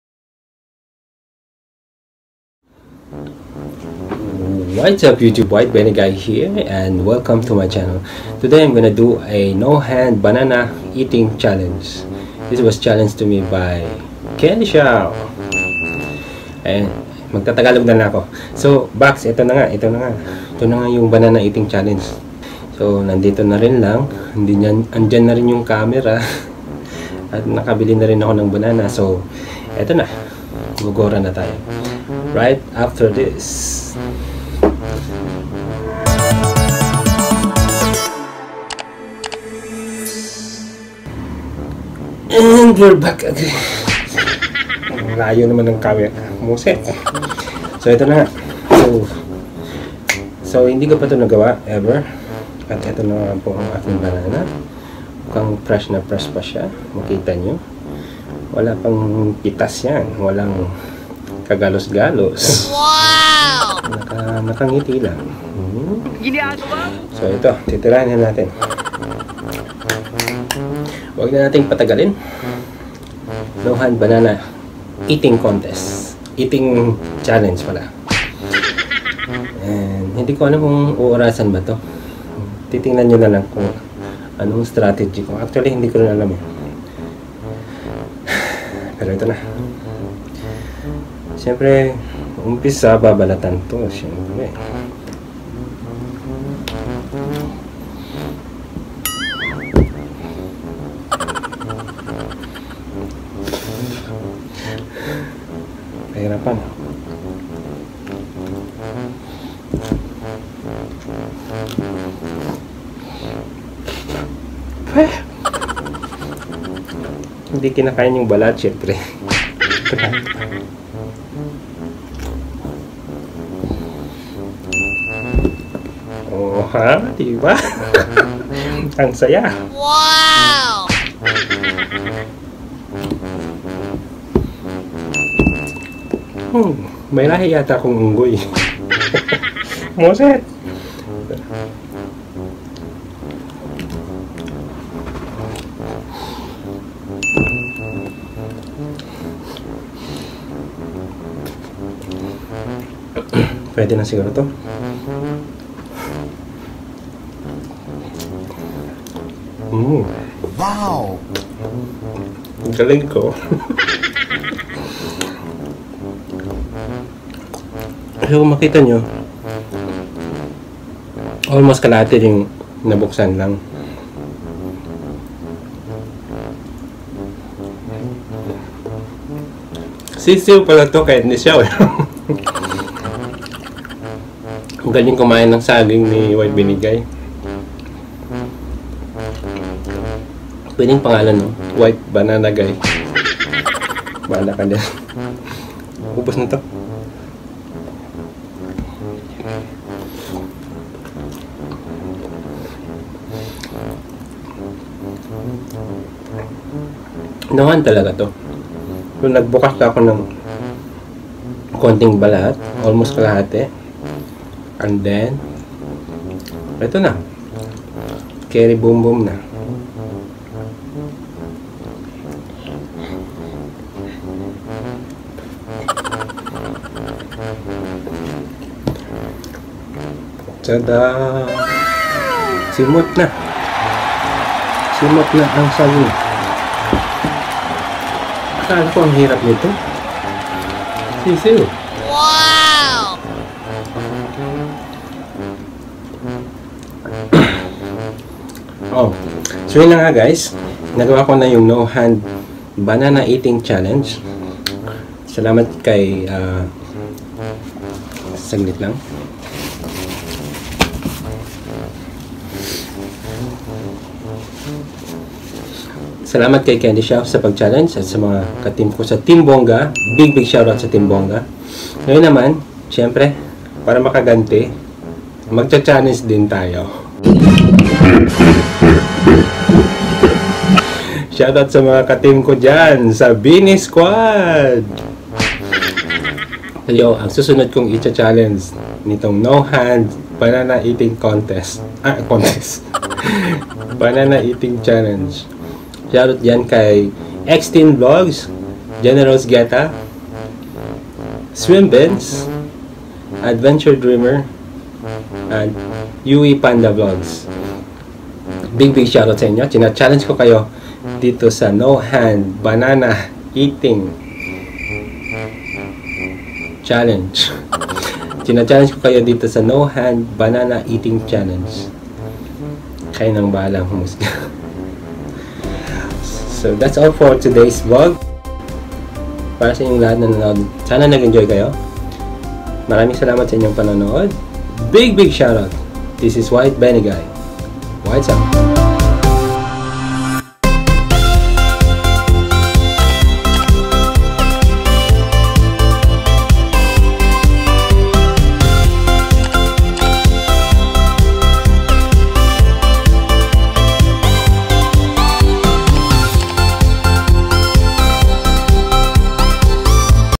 WhiteBeanieGuy takes NO HAND BANANA Eating Challenge
Challenged by @CandyHsiaoCHA, one of his teammates from Team Bongga, WhiteBeanieGuy takes Banana Eating Challenge with No Hands involved in eating.  Will he struggle?  Or will he be able to complete the challenge?  Check it out in this video! ^_^